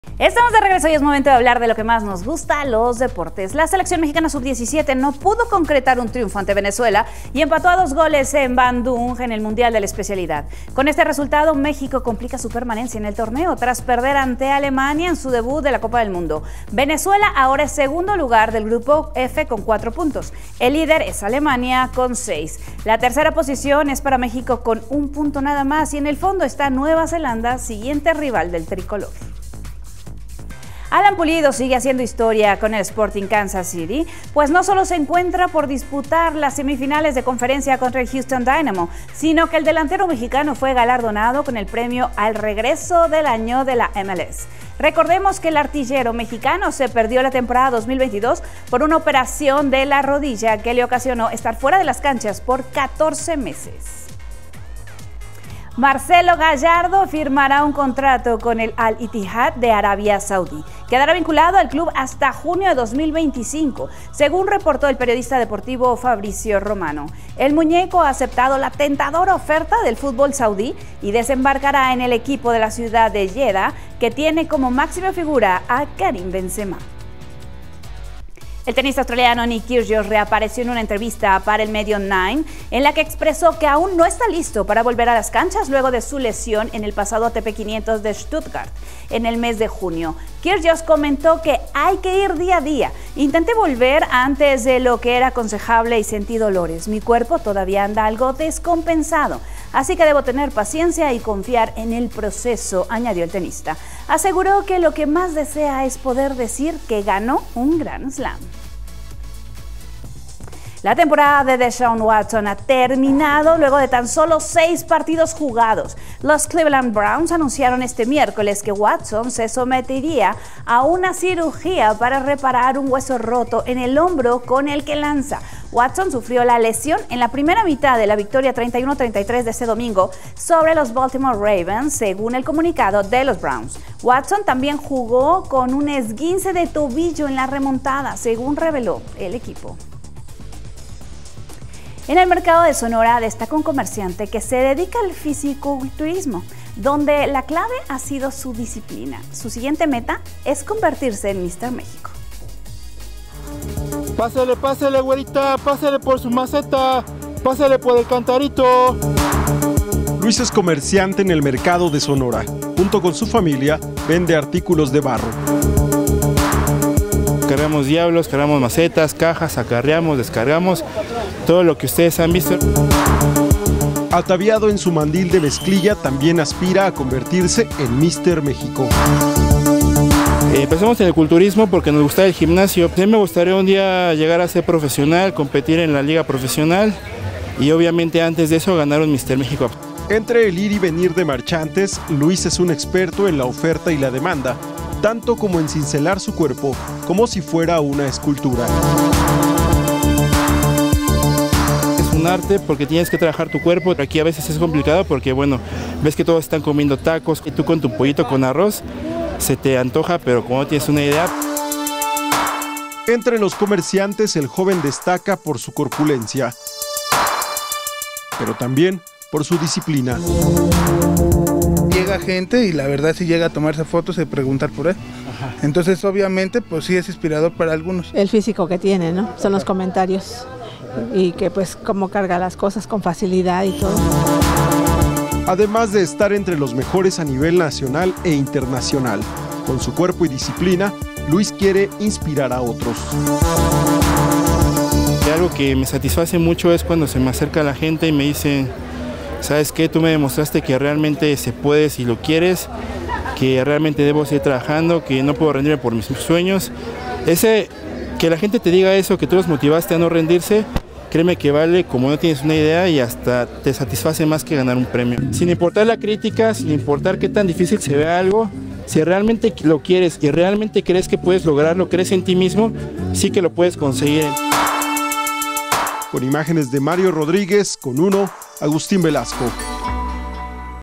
Cindy Lira nos presenta la información más importante de los deportes.
La Selección Sub-17 empató a 2 goles con Venezuela y complica su permanencia en el mundial de la especialidad.

00.00 Sub 17 empata ante Venezuela y complica su pase en la Copa del Mundo
00.31 Pulido gana el Regreso del Año en la MLS
00.56 Marcelo Gallardo dirigirá en Arabia
01.19 Kyrgios retrasará su regreso a las canchas por lesión
01.51 Deshaun Watson, fuera de la temporada de NFL por lesión
02.22 Fisicoculturista y comerciante del Mercado de Sonora